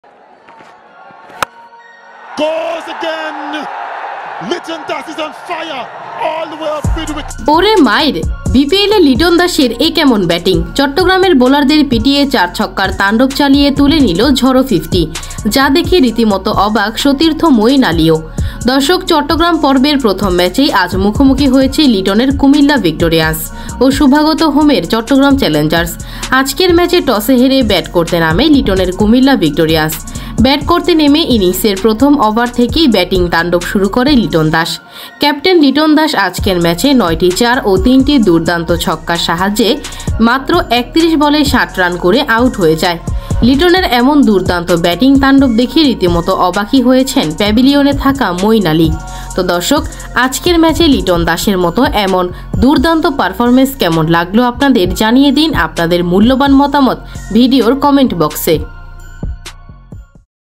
6 6 6 ওরে মাইর| চট্টগ্রামে লিটনের ব্যাটে ম্যাক্সওয়েল তান্ডব| বোলারদের পিটিয়ে ঝড়ো ফিফটি অবাক মঈন আলী
6 6 6 ওরে মাইর| চট্টগ্রামে লিটনের ব্যাটে ম্যাক্সওয়েল তান্ডব| বোলারদের পিটিয়ে ঝড়ো ফিফটি অবাক মঈন আলী